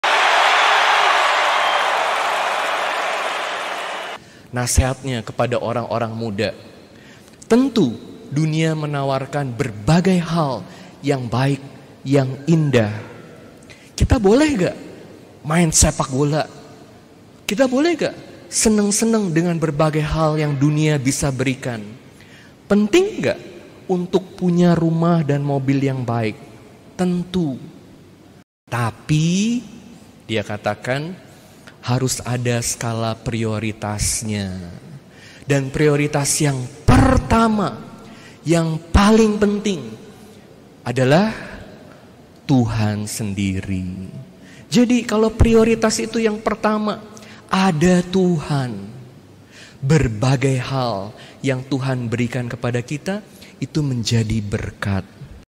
Kisah: Philip Mulryne, Pesan Untuk Kita Semua part 3
Pesan Romo Philip Mulryne OP untuk kita semua terutama kaum muda

Source: Romo Adrian Adirejo OP, Paroki Redemptor Mundi Surabaya